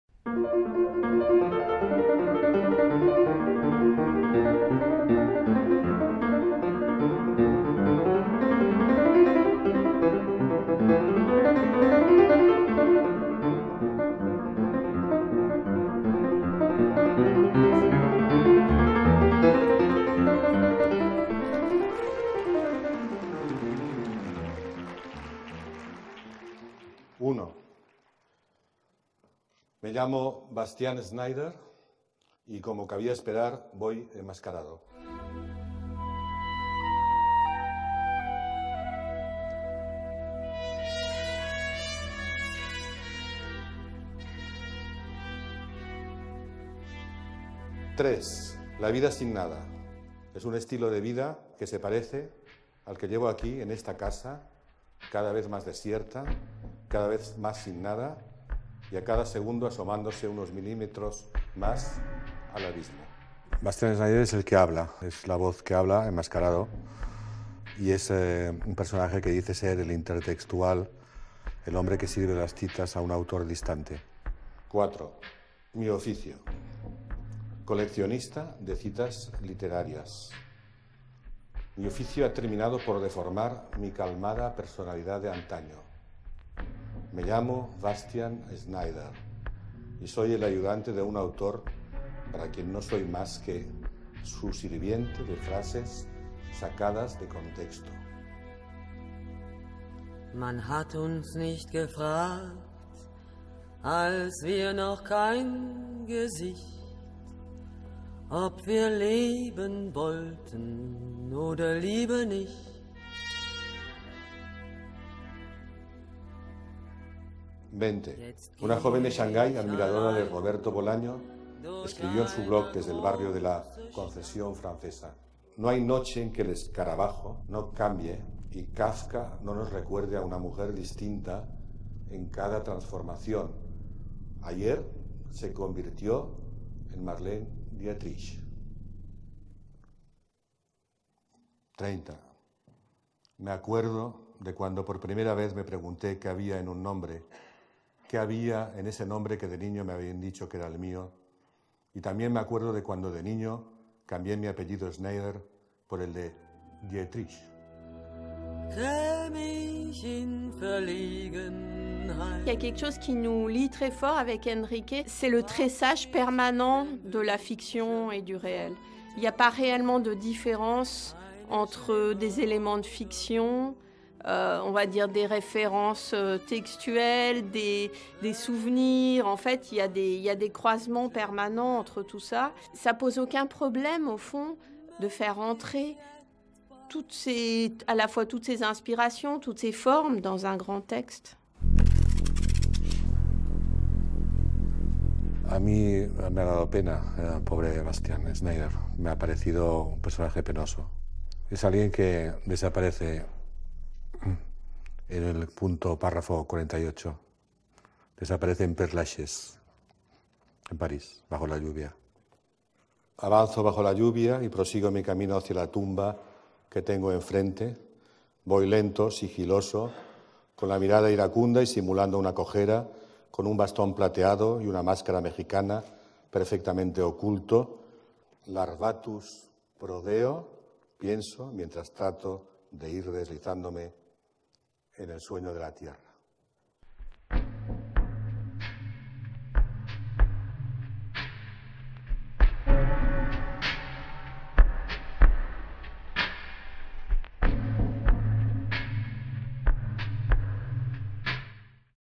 Radicalement pas original (Bastian Schneider) - Enrique Vila-Matas
L’écrivain espagnol Enrique Vila-Matas vient partager au Collège de France sa vision toute singulière de l’écriture.

Radicalement pas original (Bastian Schneider)
Extrait de la grande conférence du 24 mars 2017 avec la participation de Dominique Gonzalez-Foerster

Enrique VILA-MATAS est né à Barcelone en mars 1948. Son œuvre a été presque dans sa totalité publiée chez Christian Bourgois Editeur : Abrégé d'histoire de la littérature portative, Suicides exemplaires, Enfants sans enfants, Bartleby et compagnie, Le Mal de Montano, Paris ne finit jamais, Docteur Pasavento, Explorateurs de l'abîme, Journal volubile, Dublinesca, Perdre des théories, Impressions de Kassel, Marienbad électrique, Mac et son contretemps.
Elle a été traduite en 37 langues et couronnée par de nombreux prix littéraires : le prix Médicis étranger, le prix Rómulo Gallegos, le prix Rulfo, le prix Ennio Flaiano, le prix Elsa Morante, le prix Mondello, le prix Gregor von Rezzori, le prix Formentor, le prix national de Catalogne Chevalier de la Légion d'honneur en France, membre du convulsif Ordre des Chevaliers de Finnegans’, fondateur de la Société de "Réfractaires à l'abrutissement
général" (Nantes), et recteur (inconnu) de l'Université inconnue de New York.
Divers ouvrages critiques ont été publiés sur son œuvre ainsi qu'un livre
d'entretiens avec son traducteur français actuel.